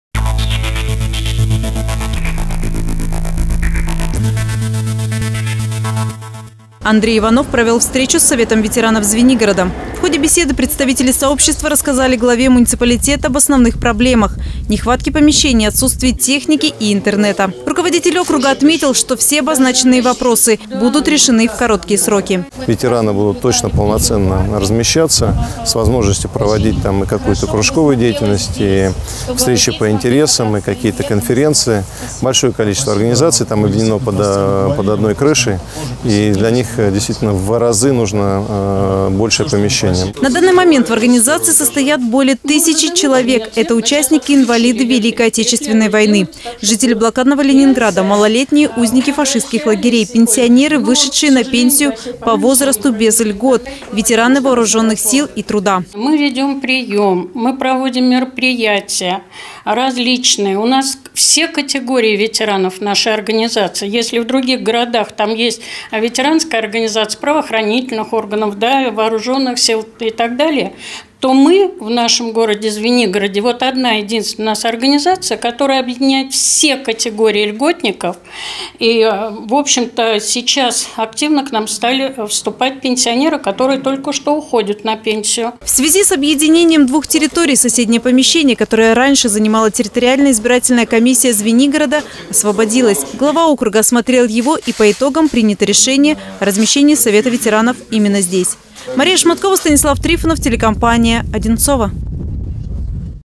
Звенигород. Совет ветеранов.
С момента объединения Звенигорода и Одинцовского района, ветеранские организации, которые находились на этих территориях -  теперь тесно взаимодействуют друг с другом. Совет ветеранов Звенигорода посетил глава округа Андрей Иванов.